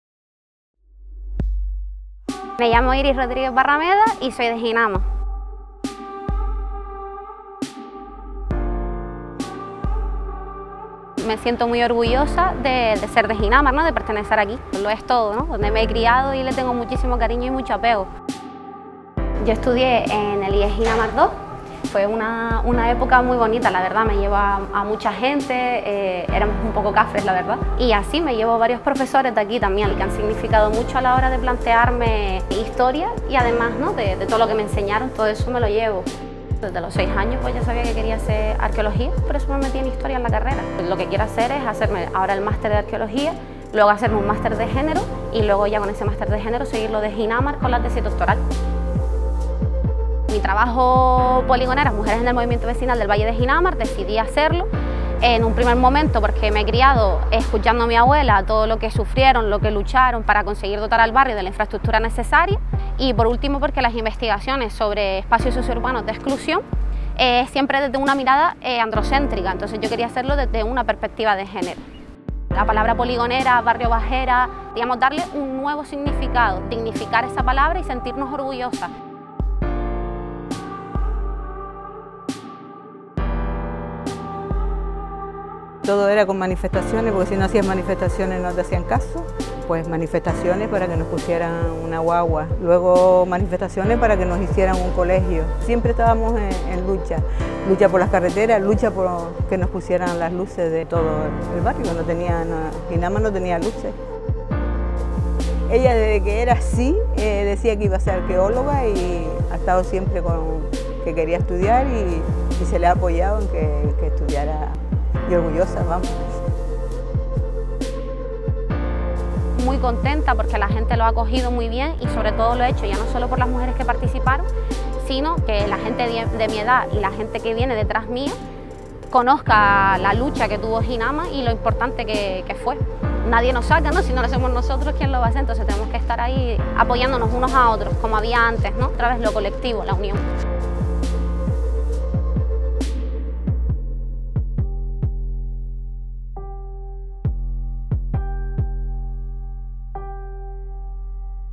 IRIS RODRÍGUEZ
Conocemos a Iris Rodríguez Barrameda, una joven de Jinámar que acaba de terminar la carrera de Historia y ha presentado su trabajo final de grado Poligoneras. Mujeres en el movimiento vecinal de Jinámar, destacando la gran importancia que han tenido las mujeres en la lucha por los derechos de este barrio.